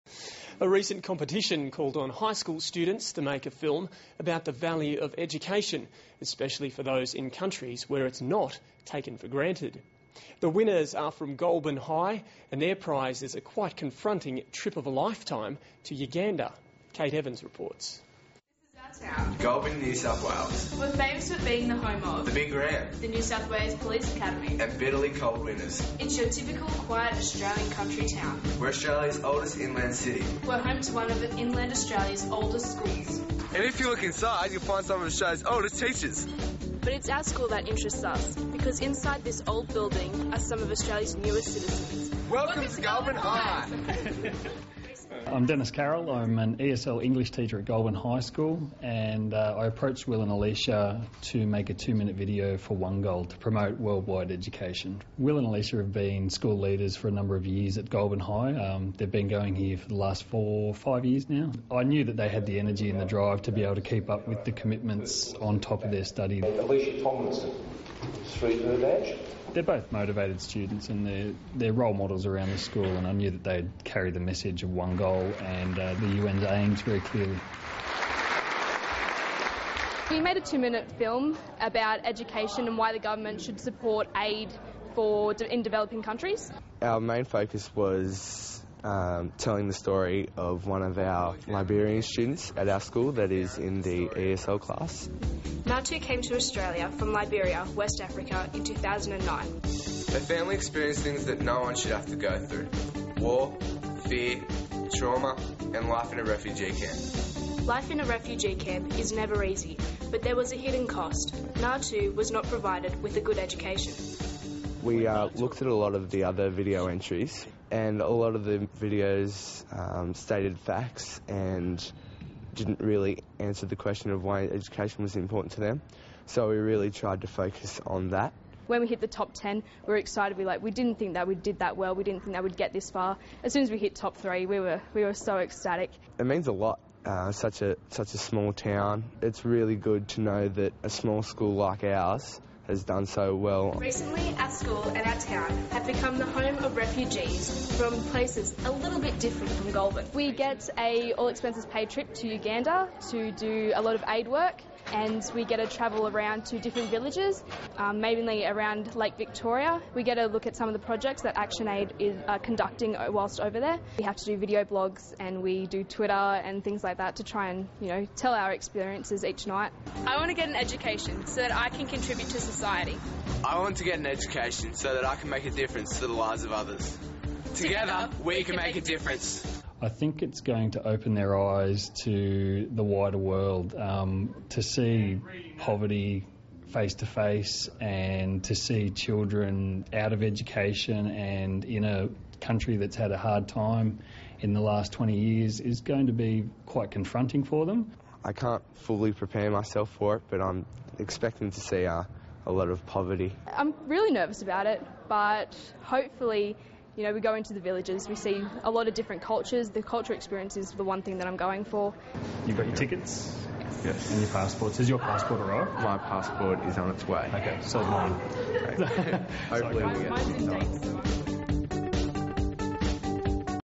Goulburn to Uganda
A recent national competition asked high school students to make a video about the value of education. The winners are from Goulburn High - and their prize is a trip to Uganda.